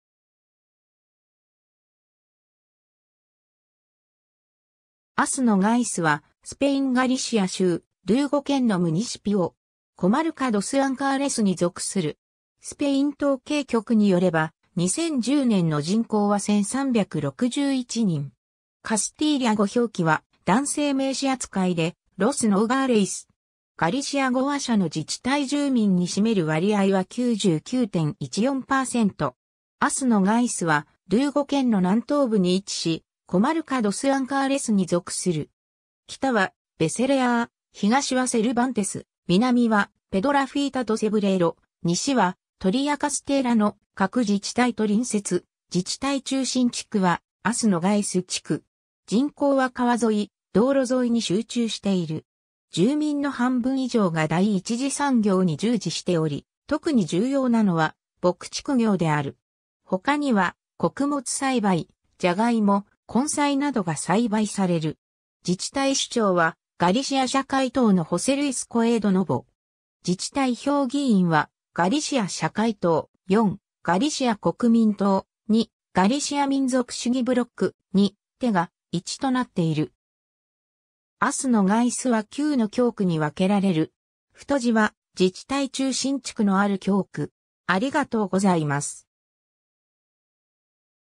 アス・ノガイス
アス・ノガイス（As Nogais）は、スペイン・ガリシア州ルーゴ県のムニシピオ（基礎自治体）。
コマルカ・ドス・アンカーレスに属する。
スペイン統計局によれば、2010年の人口は1,361人（2009年：1,381人、2006年:1,441人、2005年:1,477人、2004年:1,501人、2003年:1,520人）。
カスティーリャ語表記は、男性名詞扱いでLos Nogales（ロス・ノガーレス、ガリシア語名は女性名詞）。
 アス・ノガイスはルーゴ県の南東部に位置し、コマルカ・ドス・アンカーレスに属する。
北はベセレアー、東はセルバンテス、南はペドラフィータ・ド・セブレイロ、西はトリアカステーラの各自治体と隣接、自治体中心地区はアス・ノガイス地区。
 人口は川沿い、道路沿いに集中している。
 住民の半分以上が第一次産業に従事しており、とくに重要なのは牧畜業（牛）である。
他には穀物栽培（ライムギ、小麦、トウモロコシ）、ジャガイモ、根菜などが栽培される。
 自治体首長はガリシア社会党（PSdeG-PSOE）のホセ・ルイス・コエード・ノボ（José Luis Coedo Novo）。
自治体評議員は、ガリシア社会党：4、ガリシア国民党（PPdeG）：2、ガリシア民族主義ブロック（BNG）：2、TEGA（Terra Galega、ガリシアの大地）：1となっている。
（2007年自治体選挙結果、得票順） アス・ノガイスは9の教区に分けられる。